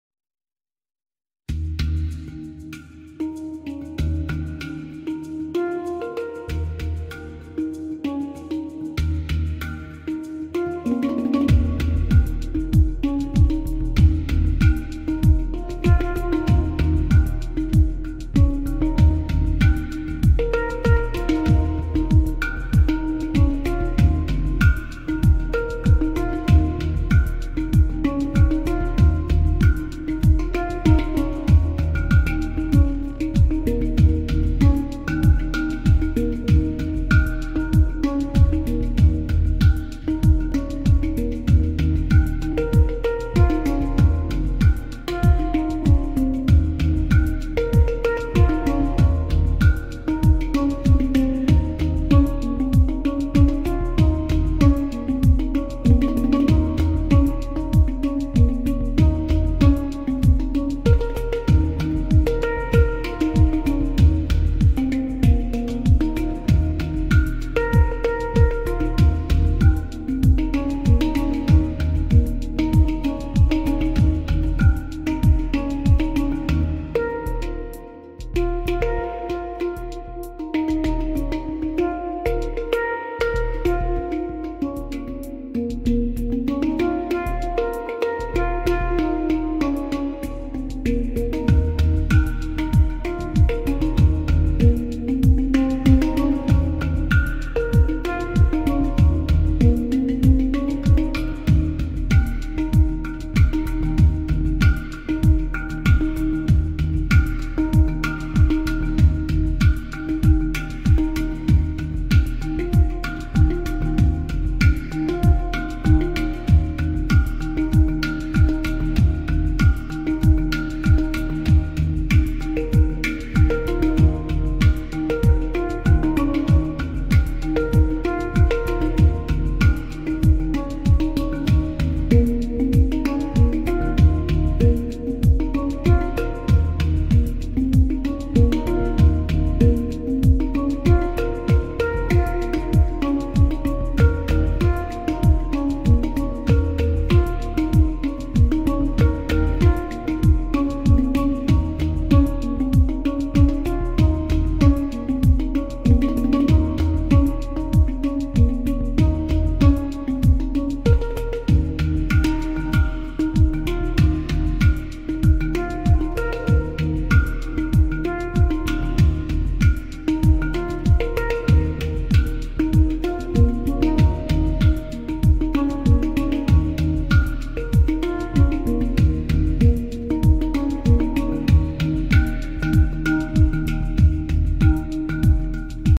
eagle predator from sky hunting snake
The eagle is a top-predator whose prey includes snakes, so it can win in battle. Enjoy watching and thank you for your support ~ hira raya new.eagle predator from sky hunting snake.